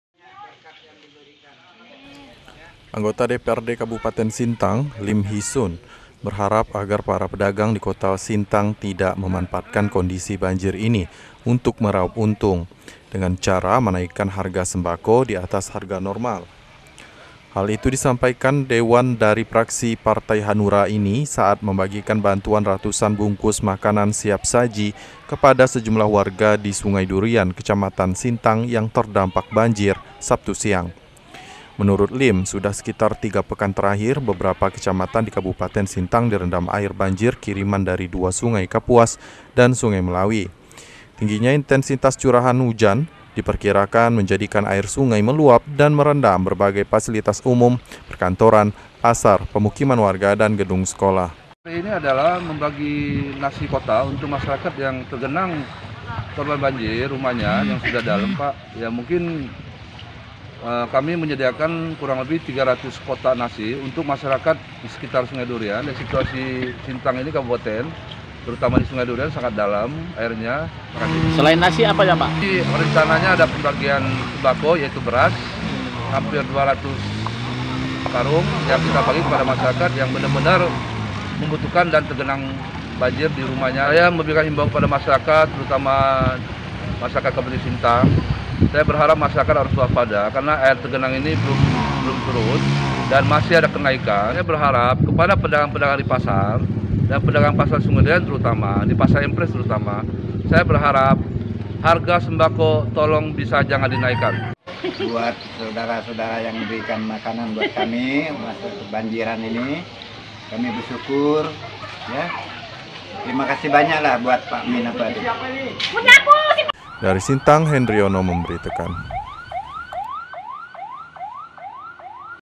PEDAGANG DIHARAP TAK NAIKKAN HARGA SEMBAKO
Para pedagang sembako di Sintang diharap tak ada yang memanfaatkan situasi dan kondisi banjir untuk meraup untung, dengan menaikkan harga barang-barang dagangannya di atas harga normal